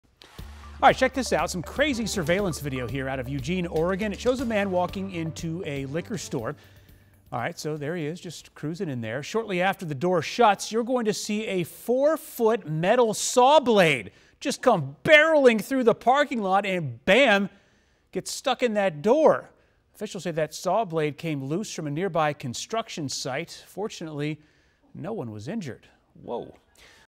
VIDEO: Man narrowly escapes runaway saw blade
Some crazy surveillance video out of Eugene, Oregon. Officials say the saw blade came loose from a nearby construction site.